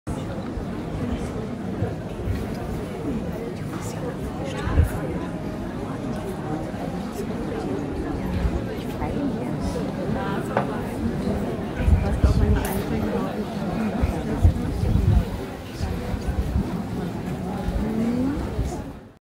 stille Figur in Wernigerode
bei jeder Spende führten die beiden dieses Bewegungen aus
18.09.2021